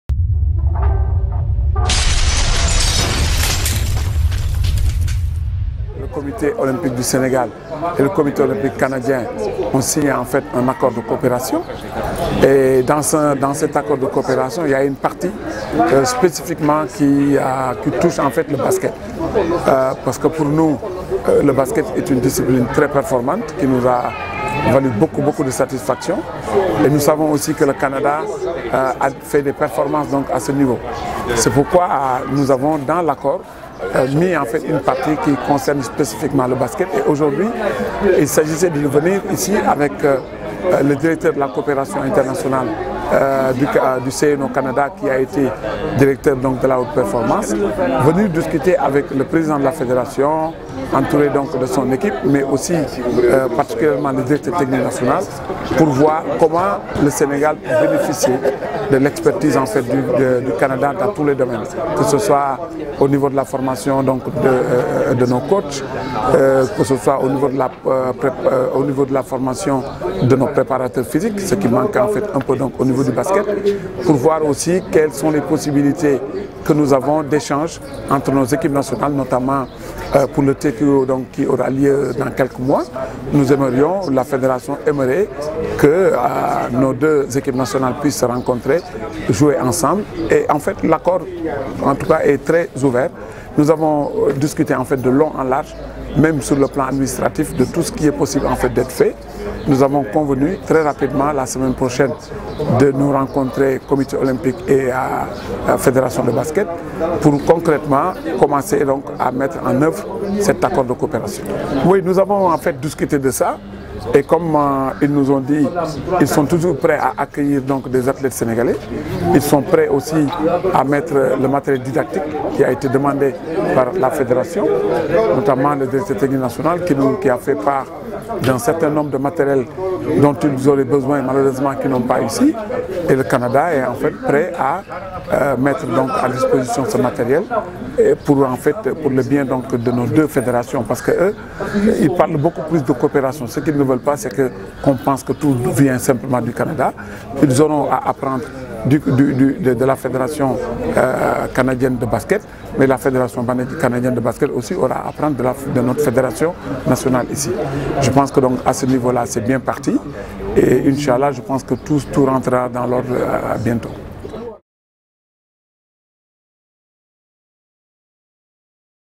Coopération Comité Olympique Canadien - FSBB : Réaction de Seydina Diagne
Mr Seydina Diagne, secrétaire général du comité olympique Sénégalais s'exprimait en marge de la réunion entre la FSBB et le comité olympique Canadien...